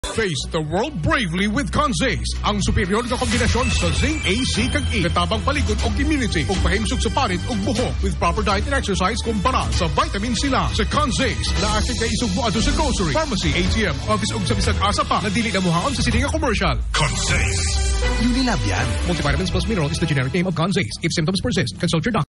Conzace Radio Ad 2022 22s (in Cebuano)
This is the Cebuano version of the 2022 Radio Ad for CONZACE.

DRPS Two 1945: Conzace Radio Ad 2022 22s (in Cebuano)